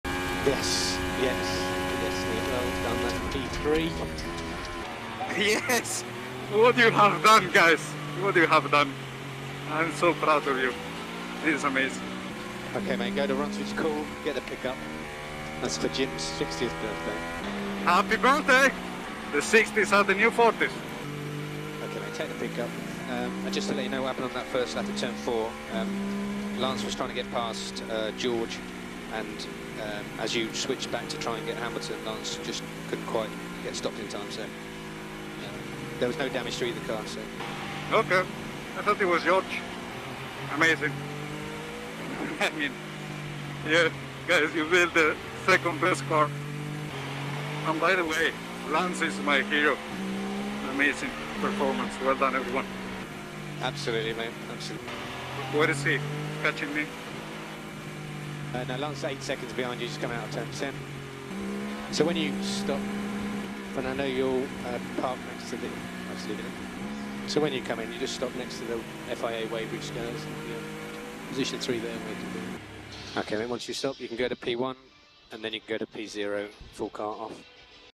Fernando Alonso's SURPRISING REACTION To Stroll's Hit At Turn 4! | F1 Bahrain GP 2023
Formula 2 Driver: Robert, KledcitoCL

Formula 3 Driver:IFM_kajus